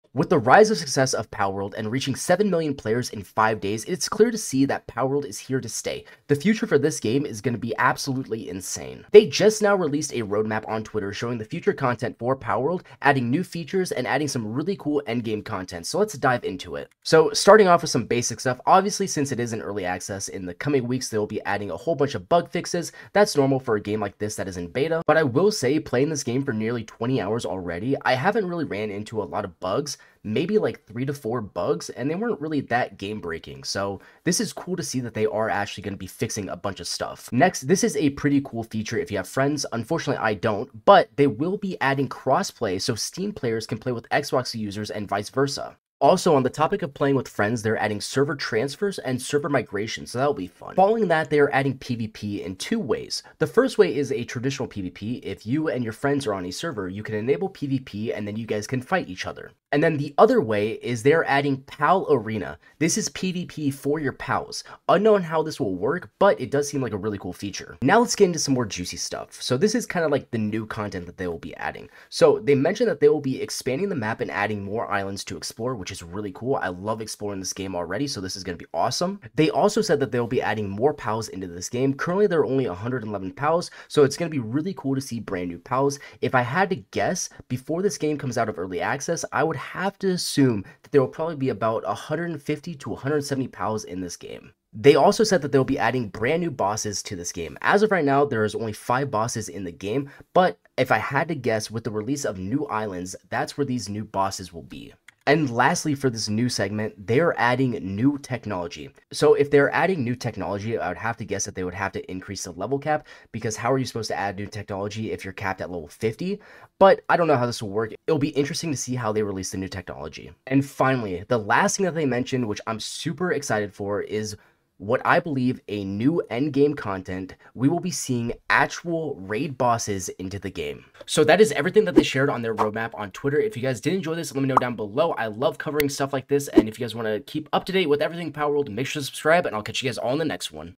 The Future For PALWORLD Is INSANE! (Huge News)
0:00 Intro
0:16 Bug fixes
0:37 Crossplay
0:46 Server transfers and migration
0:50 PvP Part 1
1:00 PvP part 2 (Pal Aerna)
1:13 Map Expansion
1:20 more pals
1:35 New Bosses
1:45 New Technology
2:01 New Raid Bosses
2:10 Outro